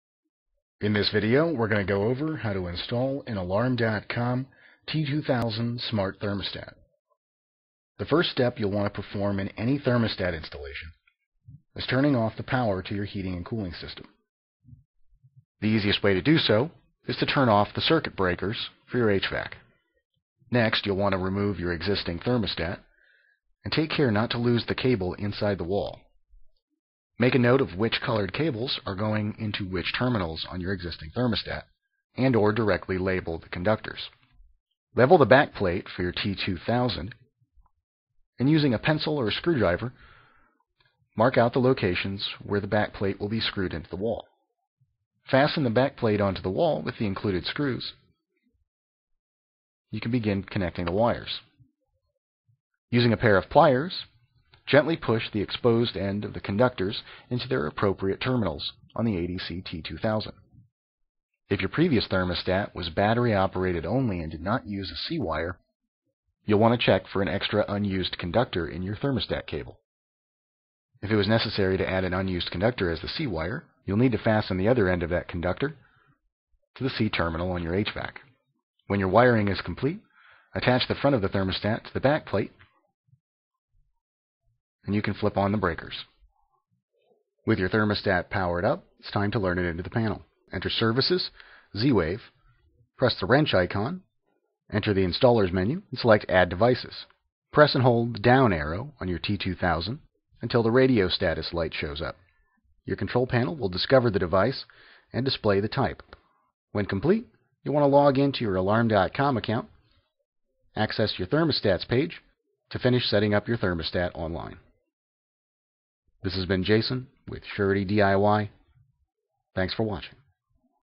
How to install an ADCT2000 Alarm.com Smart Thermostatwith a 2GIG Go!Control System
In this video we swap out an older home thermostat for the Alarm.com Smart Thermostat, giving us remote smartphone app control and notification setup to our home's heating and cooling.